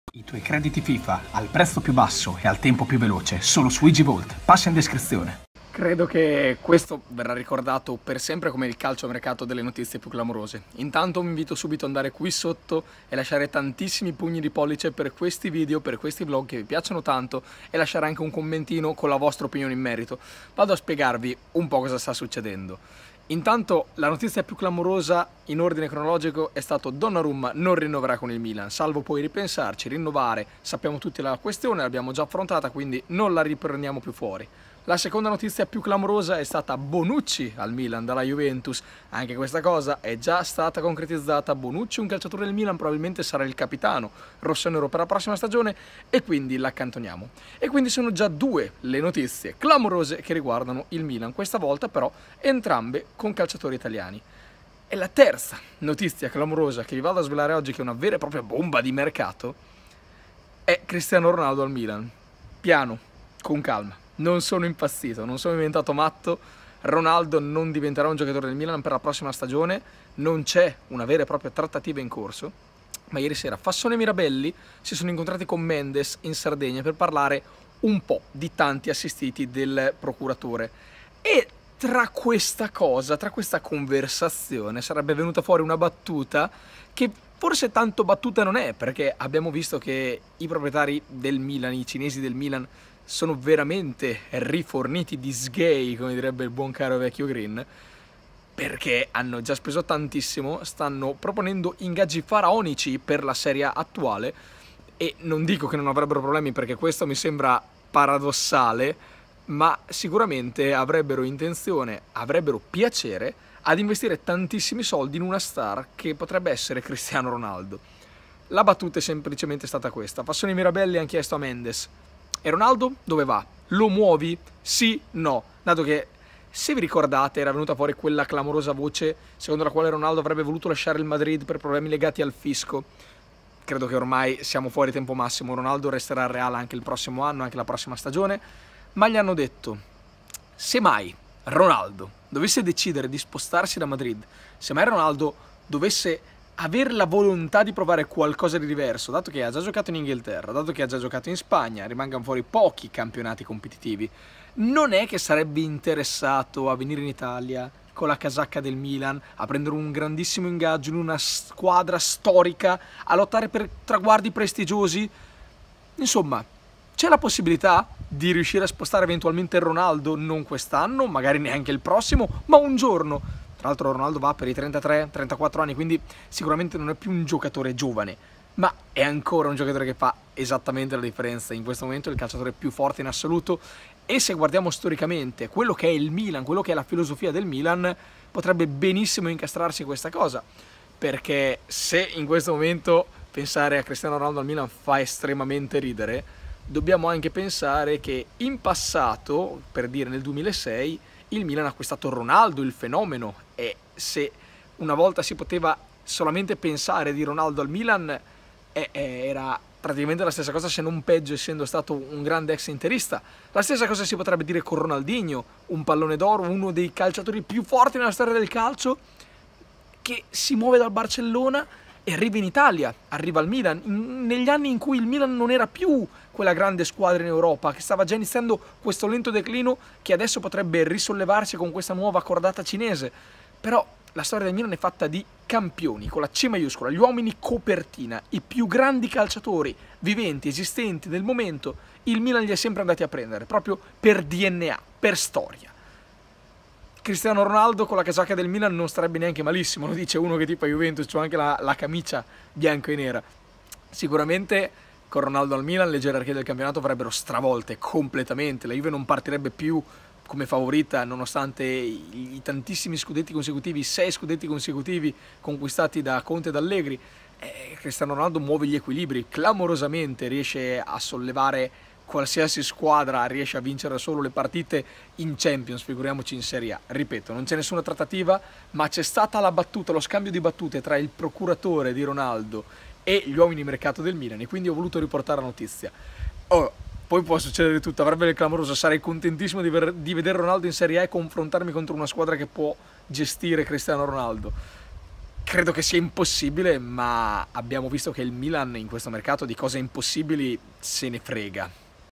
CRISTIANO RONALDO al MILAN?! FASSONE e MIRABELLI parlano all' AGENTE di CR7! [CALCIOMERCATO 2017/18]
Leggi la descrizione, va 😁